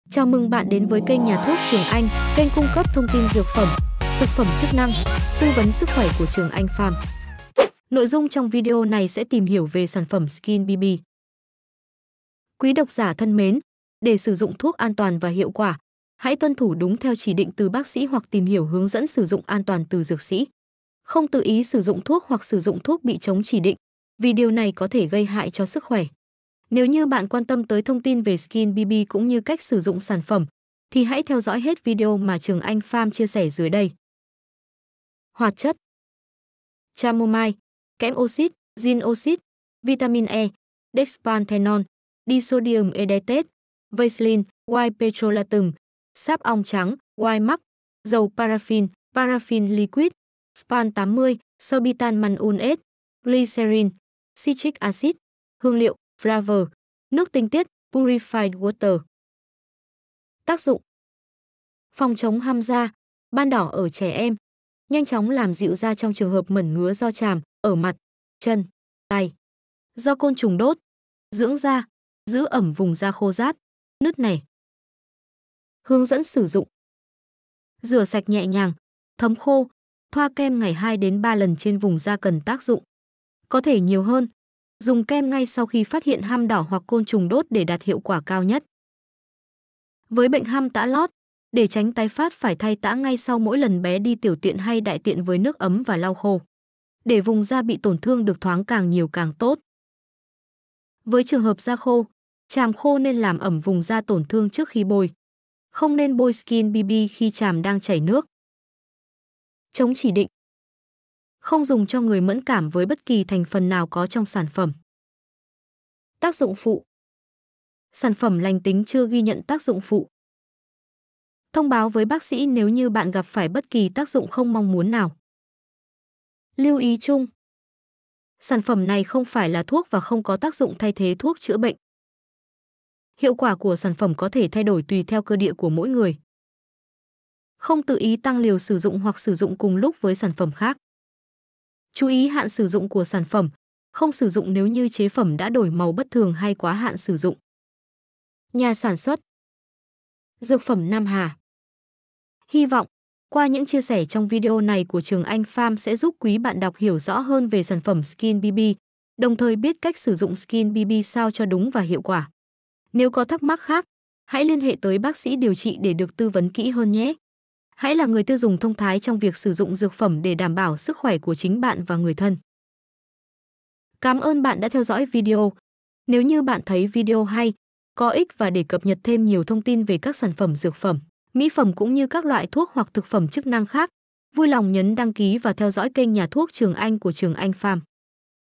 Skin Bibi - Giúp phòng chống hăm da, ban đỏ ở trẻ em
Công dụng
Phòng chống hăm da, ban đỏ ở trẻ em. Nhanh chóng làm dịu da trong trường hợp mẩn ngứa do chàm (ở mặt, chân, tay...), do côn trùng đốt. Dưỡng da, giữ ẩm vùng da khô rát, nứt nẻ.

Nội dung video được cung cấp bởi Trường Anh Pharm